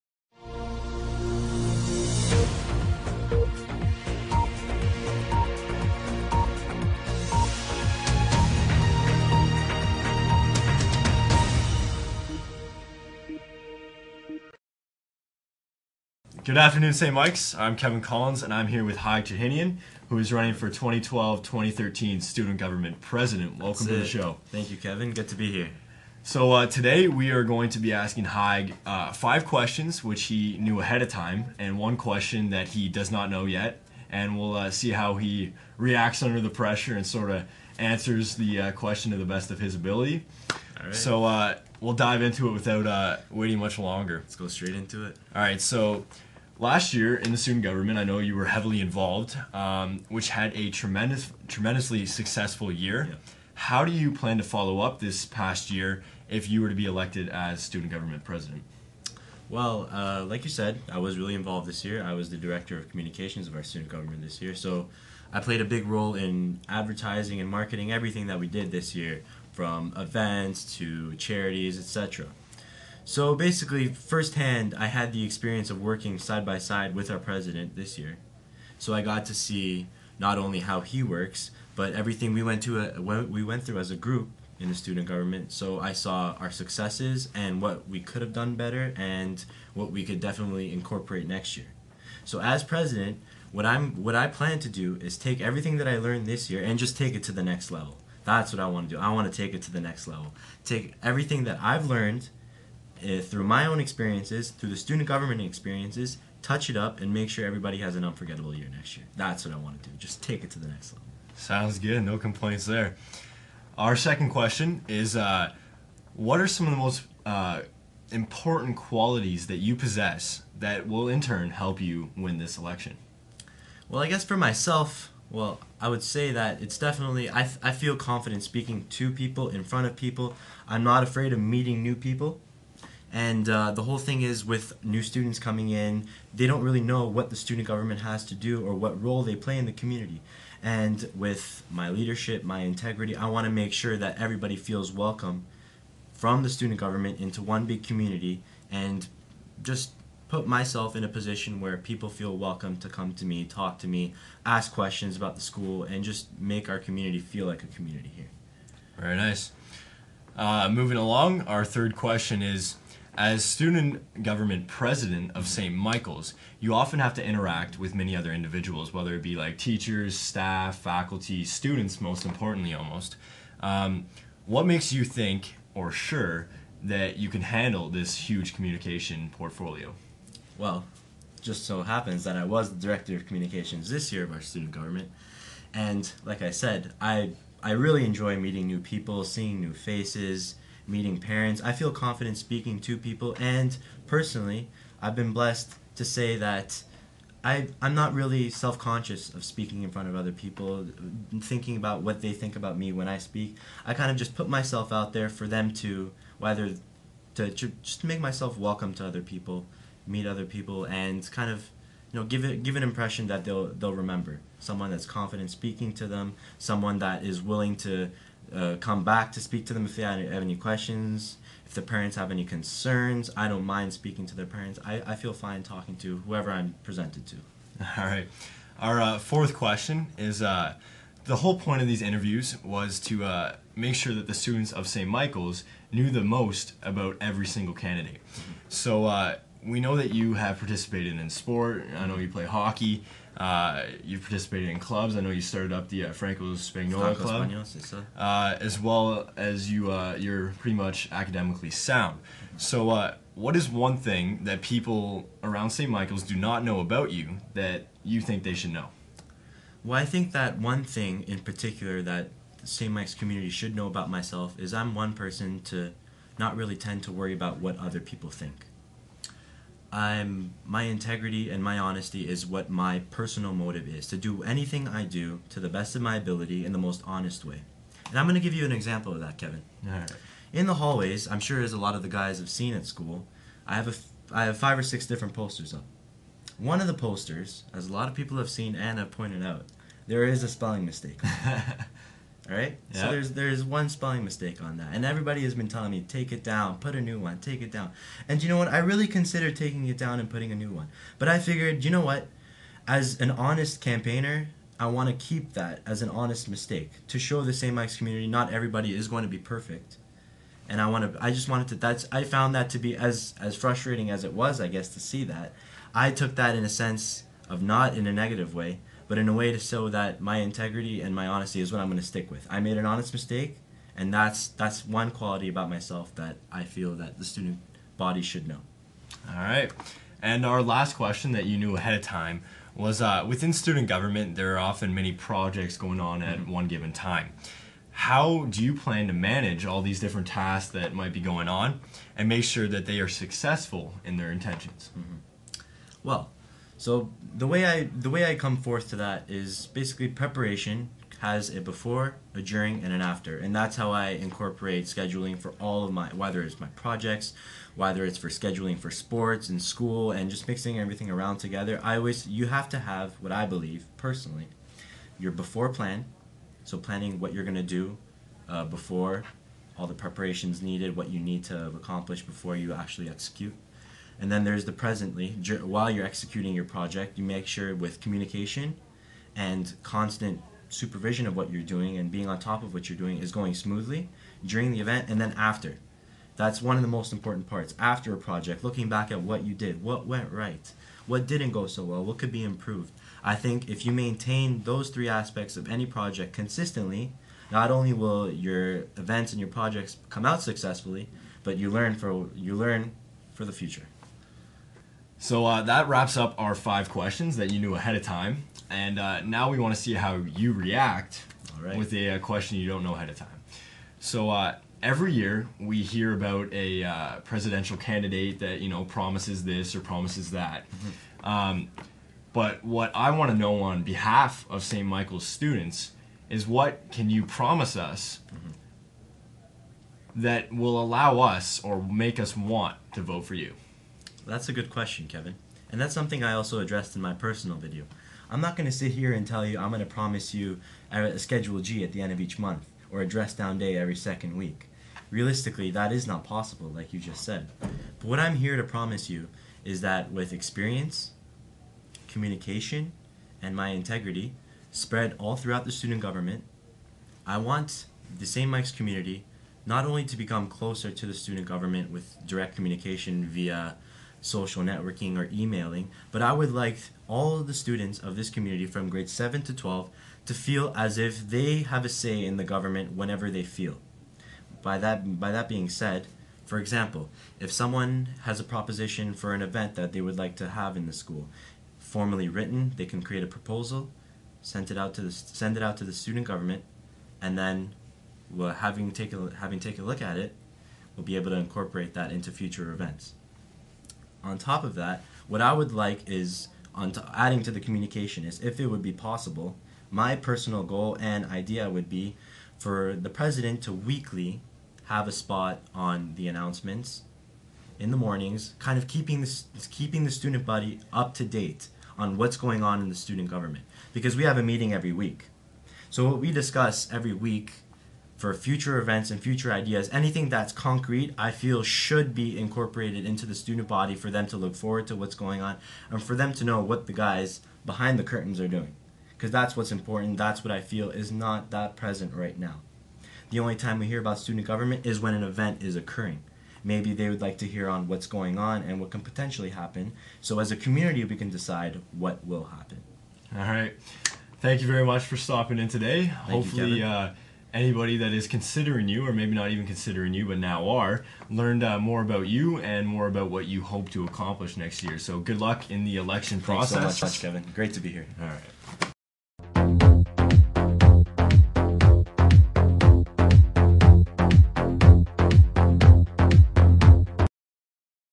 Candidate for SMCS President Haig Chahinian
Haig is running for St. Michael's Student Government President positon. Interview hosted by Kevin collins